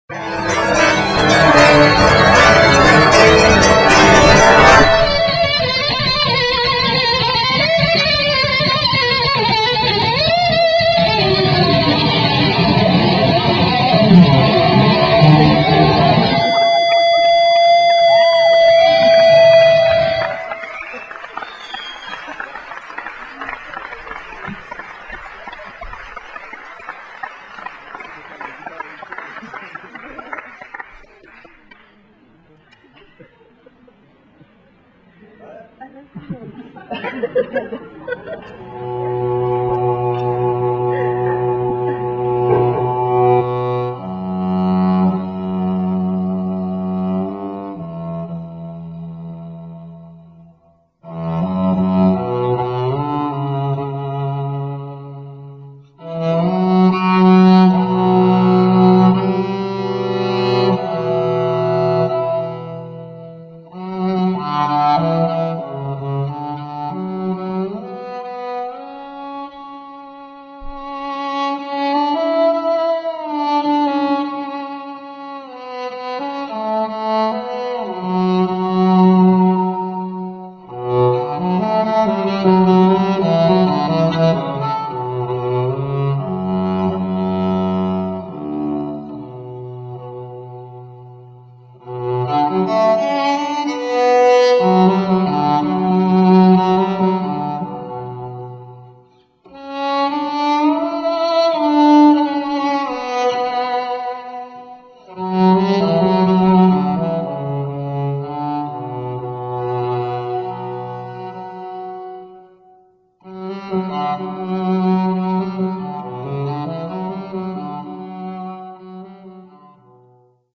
la cello de mazette..
le groupe mazette est devenu un trio,la nouvelle recrue est une violoncelliste qui se nomme pascale berthomier..ce petit film la présente avec humour et tendresse..